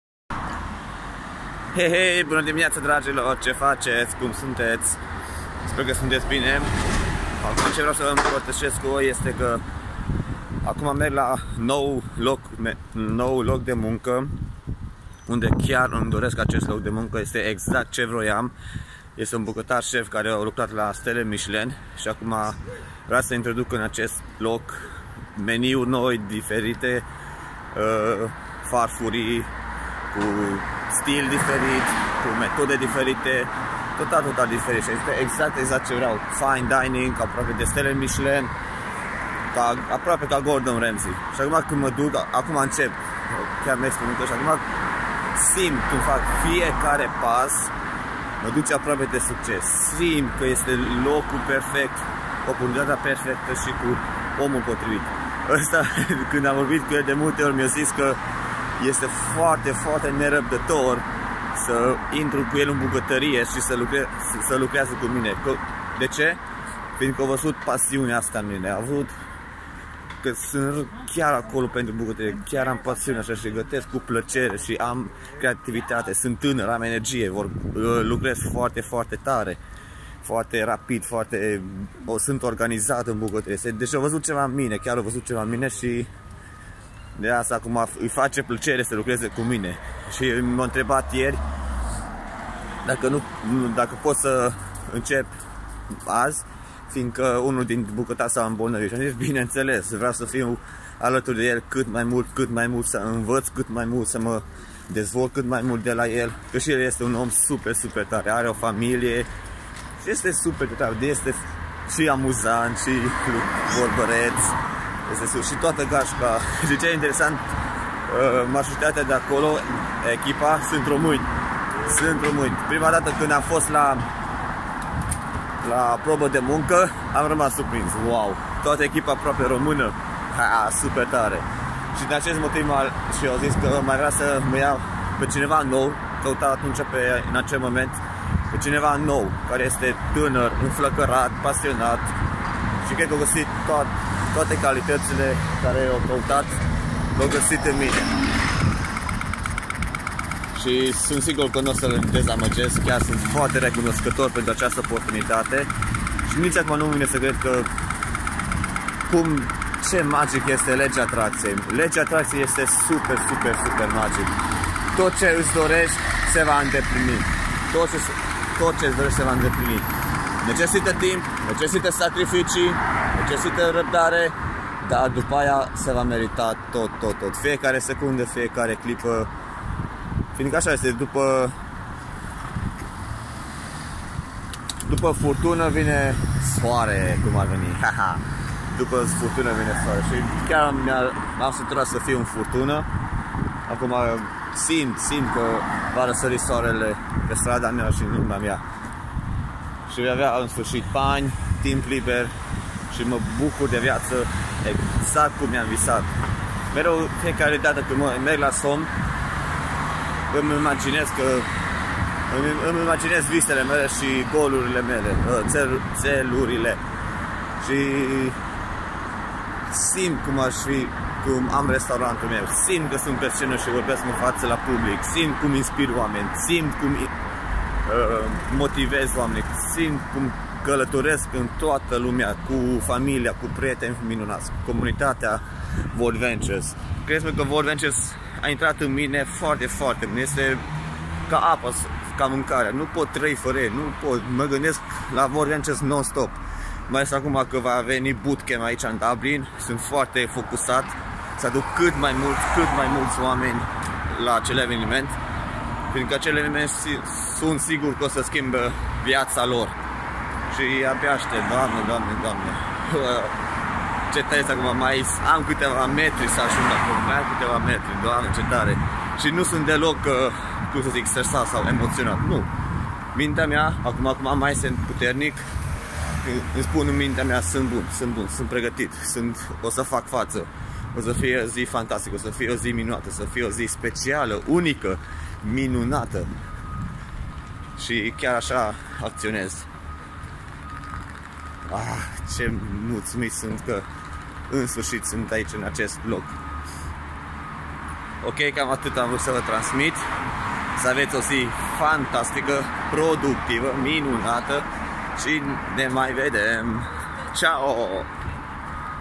Nou loc de munca.
Ati avut senztia aceea cand simtit pur si simplu cum faci fiecare pas spre o oportunitate nou care stii ca iti va afecta viitorul si ca esti foarte aproape de succes ? Va rog frumos sa lasati raspunsul vostru in sectiune de comentarii. Va multumesc foarte frumos.
Este uimitor cum functioneaza legea atractiei orice iti doresti in viata vei atrage doar trbuie sa ai rabdare,vizuine,dorinta, si incredere si se va intampla. Lucruruile marete ce se va intampla in viata noastra necesita timp, succesul este un maraton, nu un sprint.